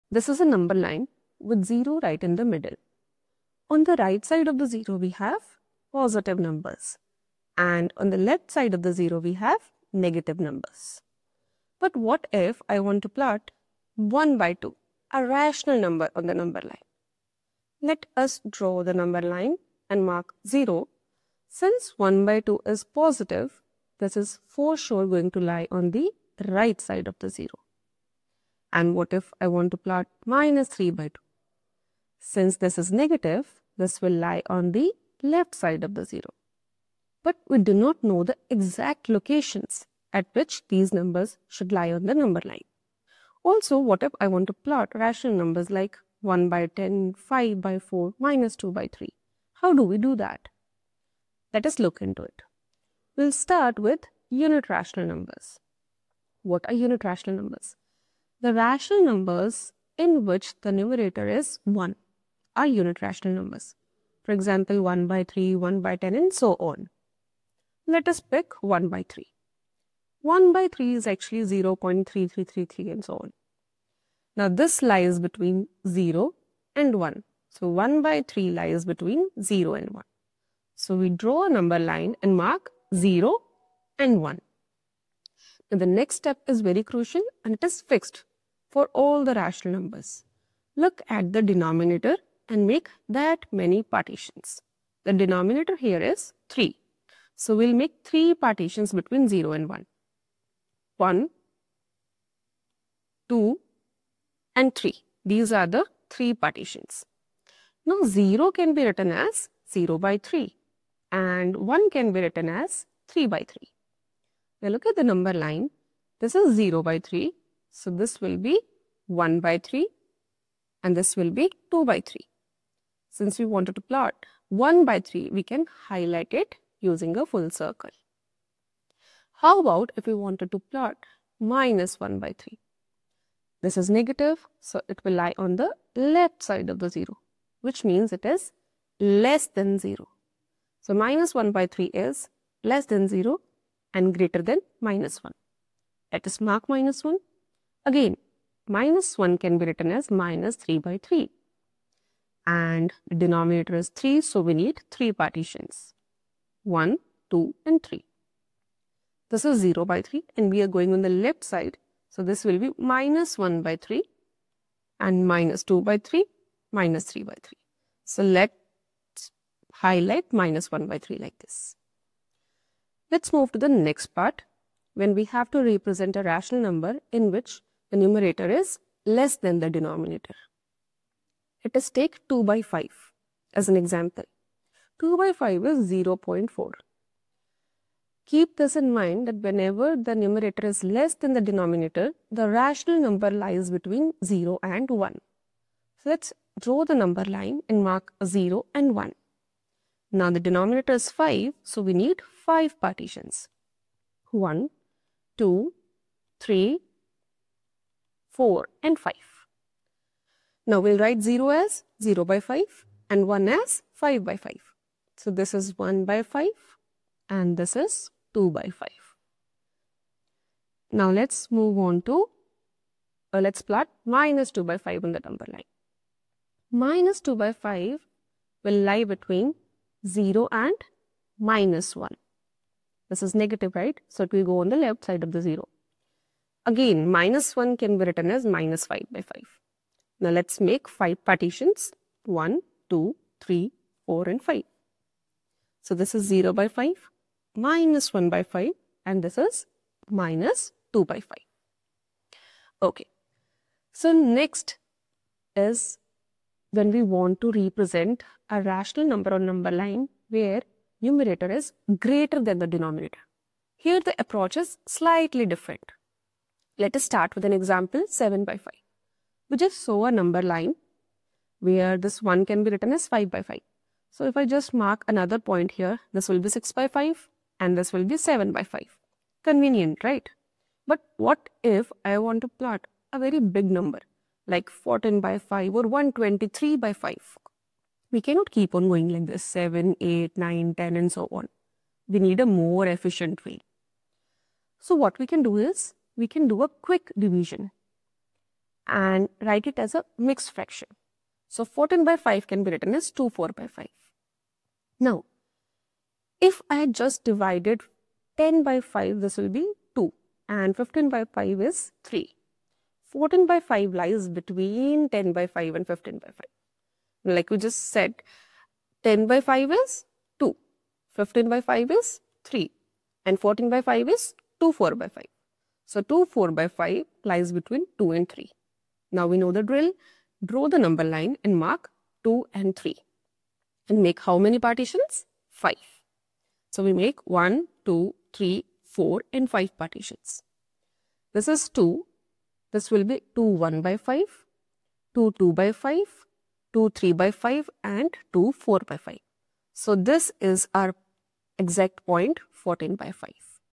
Plotting rational numbers on a number line | NCERT Grade 7 | MATH | Khan Academy
Step into the world of rational numbers and learn how to plot them on a number line like a pro! From unit fractions to numbers where the numerator is smaller and greater than the denominator, this video breaks down all the cases with clarity. Perfect for leveling up your math game!

Courses on Khan Academy are always 100% free. Start practicing—and saving your progress—now!

Timestamps:
0:00 Introduction
00:55 Plotting unit rational numbers
02:55 Plotting rational number where numerator is smaller than denomination
04:15 Plotting rational number where numerator is greater than denomination

Created by Mahak Wasan